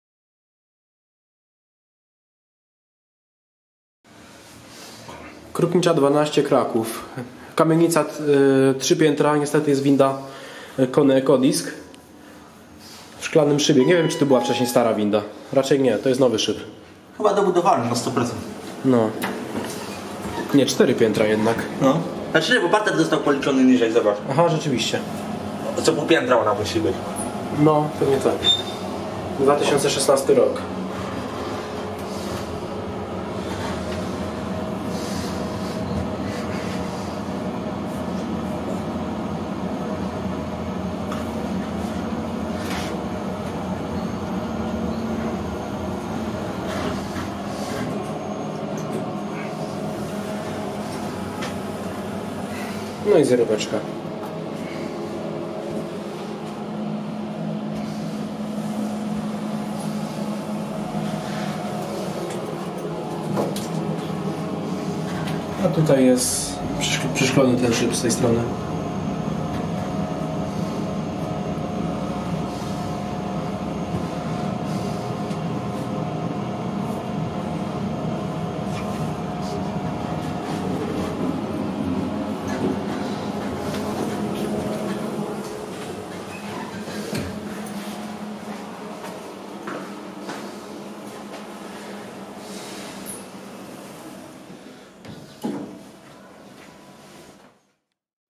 KONE Ecodisc z 2016 / 2016 KONE Ecodisc @ Krupnicza 12, Kraków (Cracow, Poland)
Nagrywane z Karolem1994
Powiedziałem "niestety", bo spodziewaliśmy się tu przedwojennego dźwigu ;p